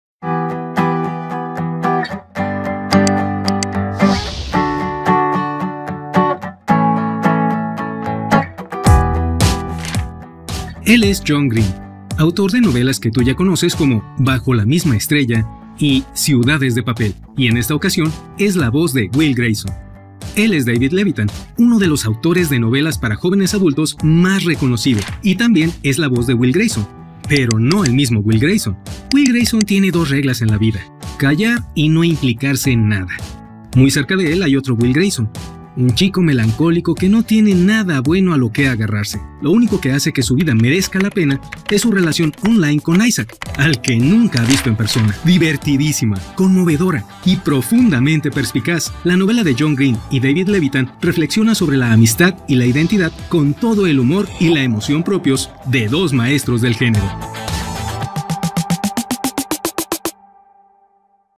WILL GRAYSON, WILL GRAYSON de John Green y David Levithan
Divertidísima, conmovedora y profundamente perspicaz, la novela de John Green, autor de Bajo la misma estrella, y David Levithan reflexiona sobre la amistad y la identidad con todo el humor y la emoción propios de dos maestros del género.

Dos adolescentes con el mismo nombre, en círculos sociales distintos, encontrarán la culminación de sus vidas entrelazadas en el más fabuloso musical a punto de estrenarse en el escenario de su escuela...